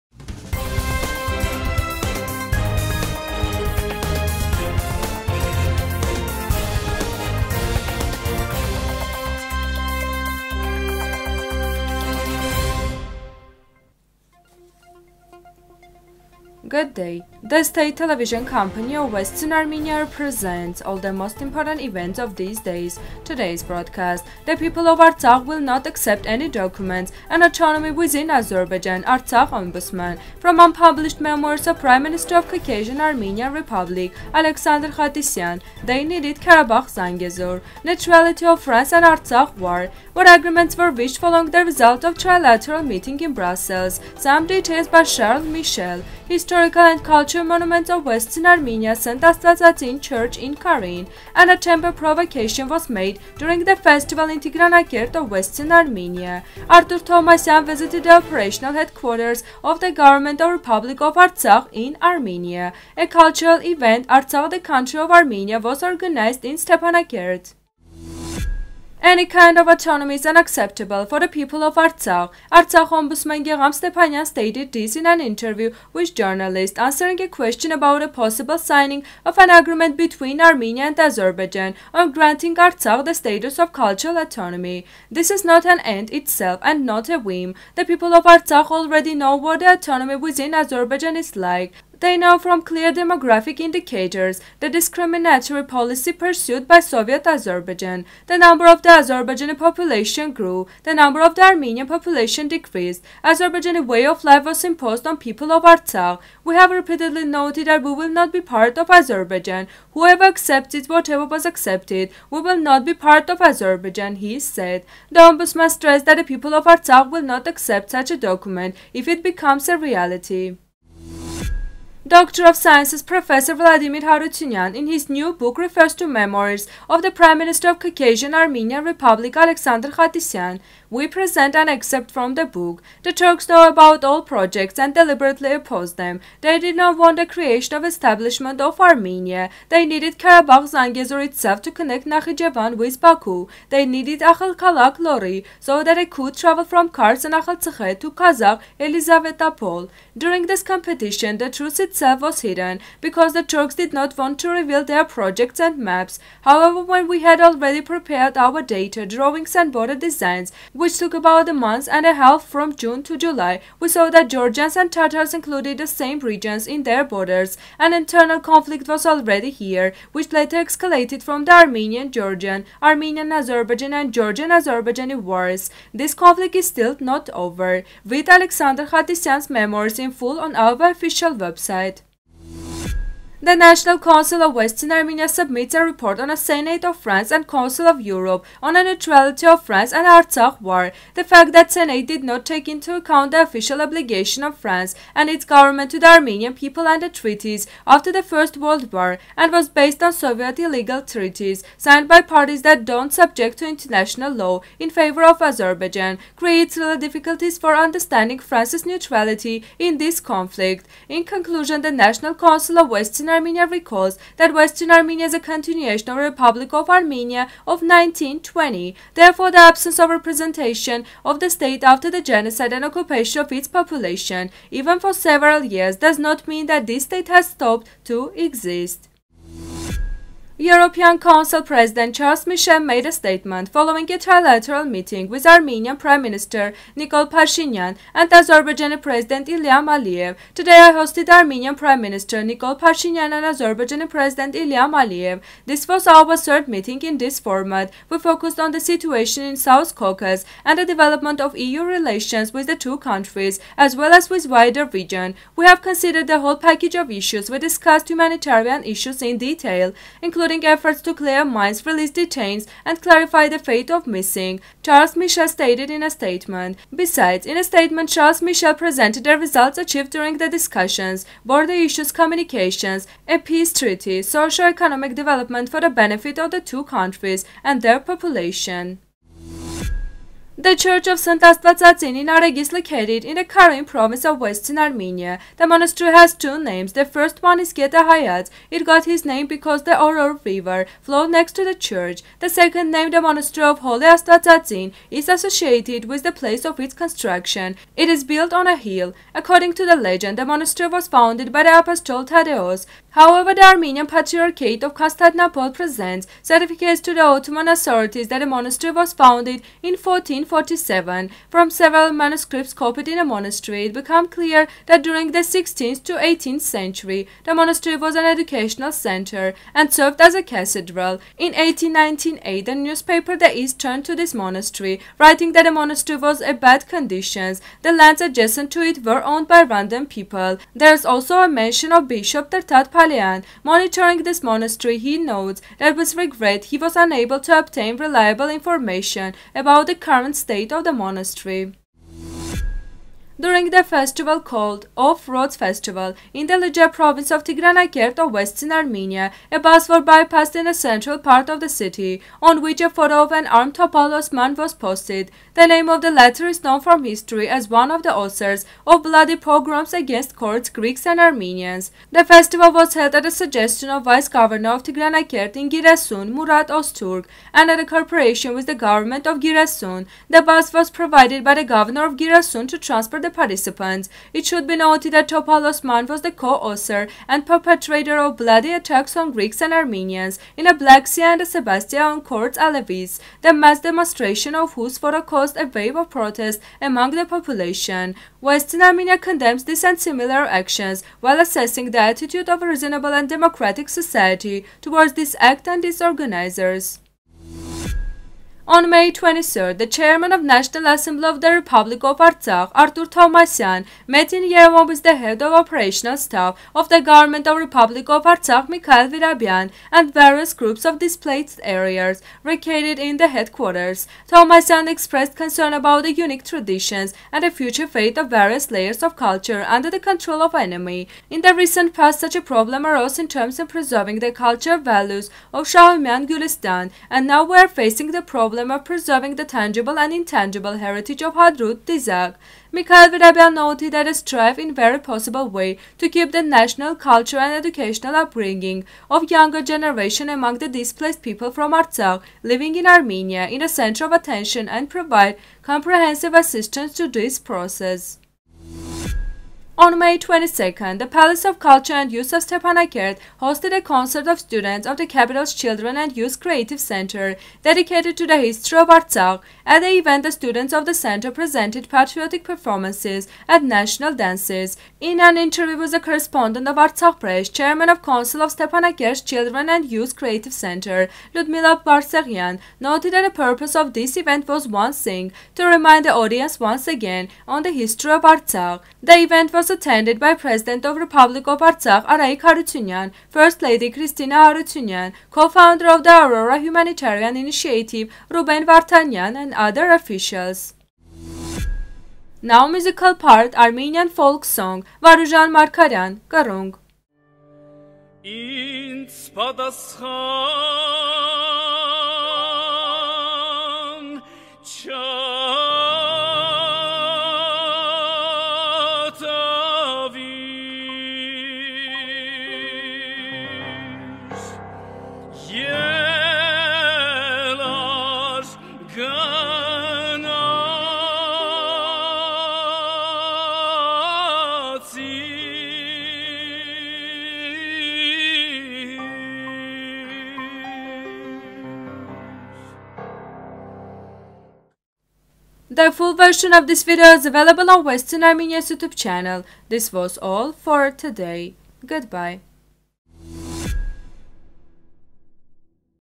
Historical and cultural monuments of Western Armenia - St. Astvatsatsin Church in Karin.
1. The people of Artsakh will not accept any documents on autonomy within Azerbaijan. Ombudsman.
2. From unpublished memoirs of the Prime Minister of the Caucasian Armenian Republic Alexander Khatisyan: "they needed Karabakh-Zangezur".
3.  Neutrality of France and the Artsakh War.
4. What agreements were reached following the results of the trilateral meeting in Brussels? Some details by Charles Michel.
5. Historical and cultural monuments of Western Armenia - St. Astvatsatsin Church in Karin.
6. An attempt of provocation was made during the festival in Tigranakert of Western Armenia.
7. Artur Tovmasyan visited the operational headquarters of the Government of the Republic of Artsakh in Armenia.
8. A cultural event "Artsakh, the country of Armenia" was organized in Stepanakert.